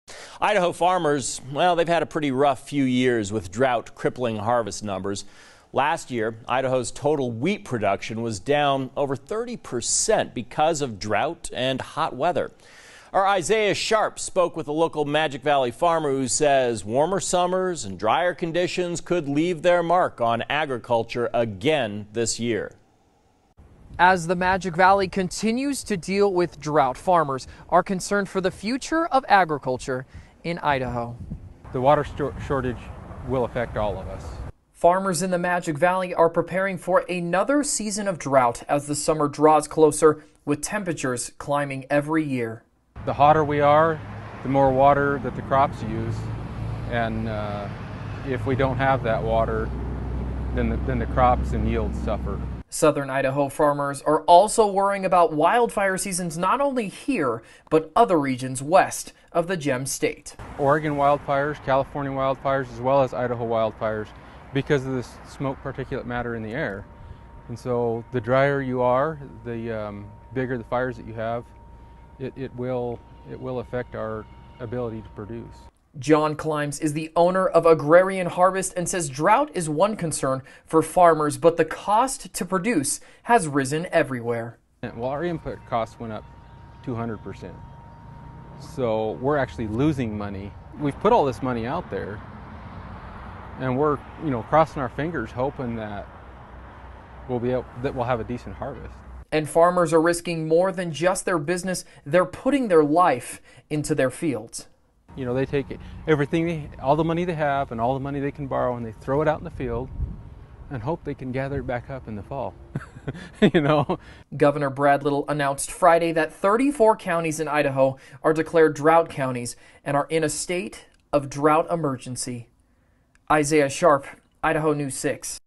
Drought causing concern for Magic Valley farmers
Farmers in the Magic Valley are concerned for the coming summer months as Idaho continues to battle drought.